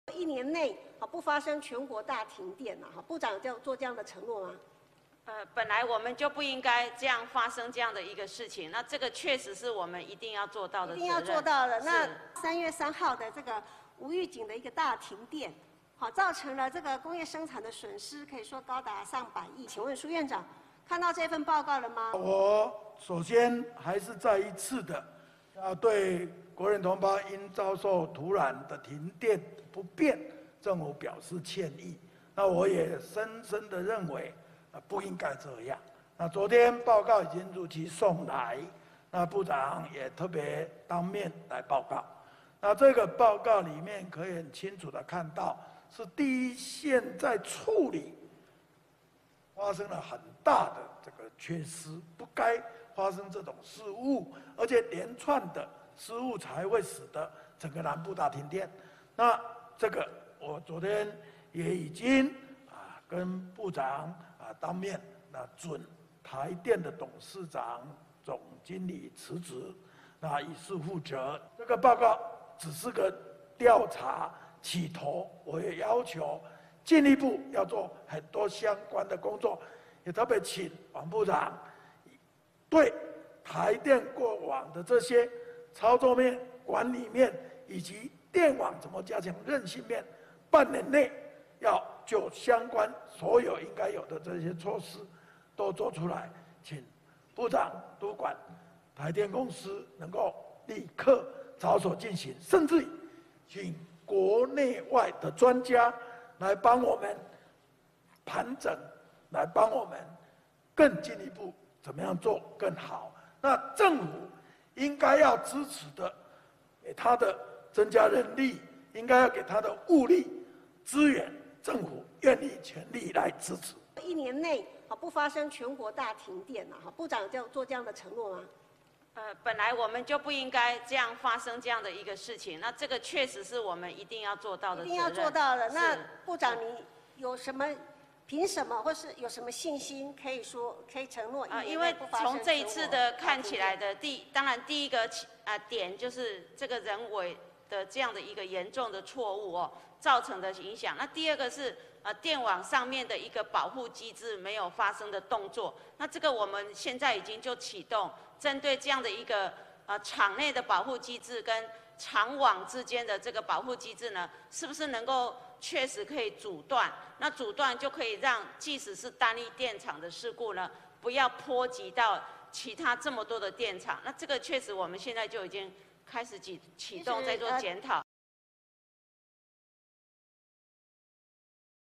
303大停電蘇貞昌再道歉 王美花：保證不再停電
303全台大停電，經濟部向行政院提出「303停電事故檢討報告」，行政院長蘇貞昌今(8)日表示，對國人遭受停電表示歉意，深深認為不該這樣，台電董事長辭職以示負責，他也准辭；請經濟部長王美花督管台電公司，並請國內外專家盤整，進一步做到最好。王美花也表示，一定要做到不再停電。